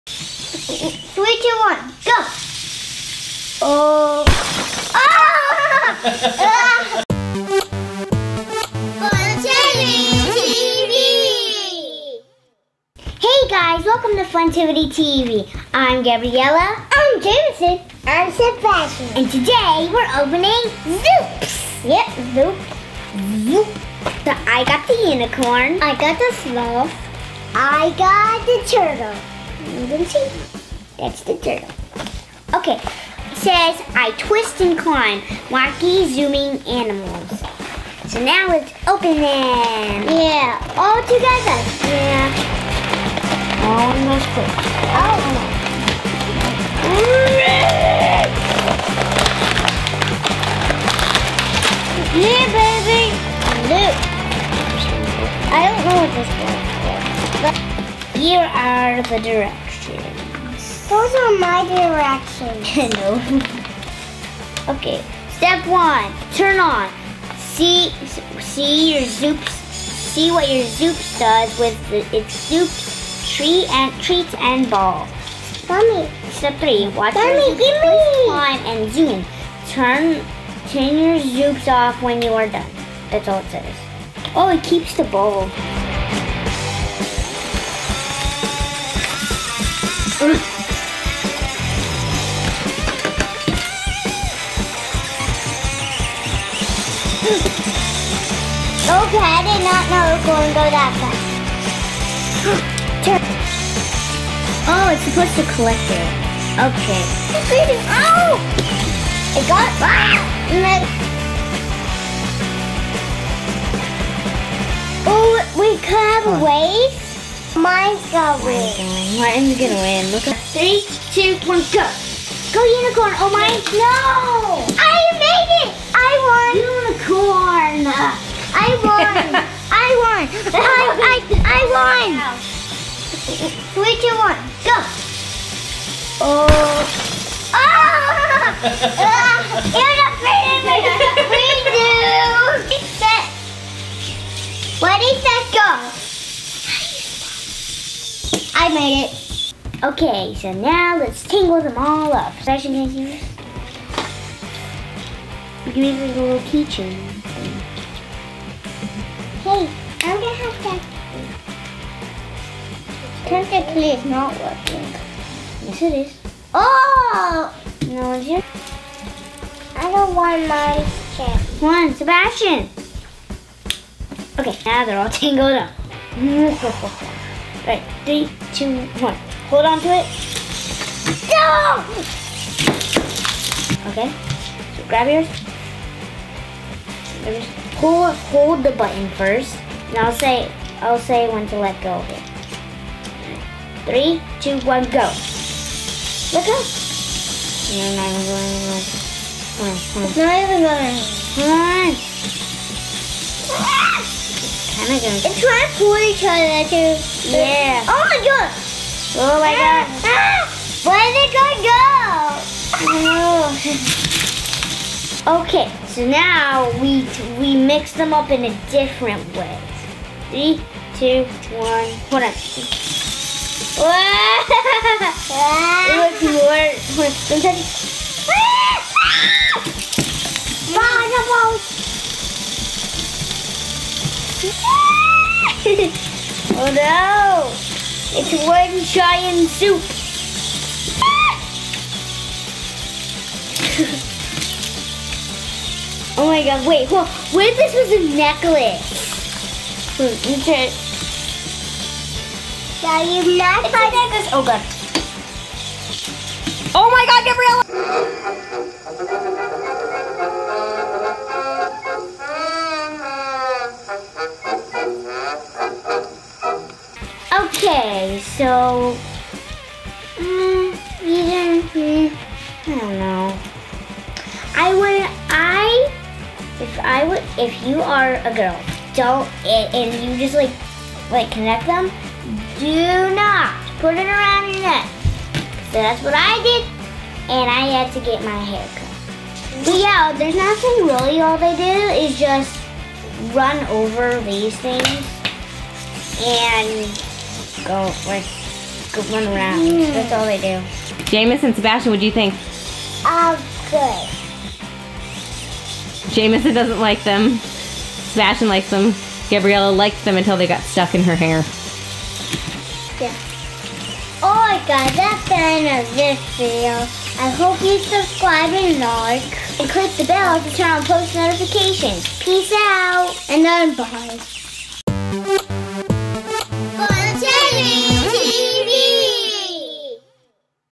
NEW HASBRO ZOOPS TOY UNBOXING AND REVIEW - GETS STUCK IN GABRIELLA'S HAIR!
Gabriella, Jameson and Sebastian unbox and review Zoops, a new toy by Hasbro. This silly toy is met with mixed reviews from the kids. Gabriella learns quickly not to put it around her neck while on or it gets stuck in your hair!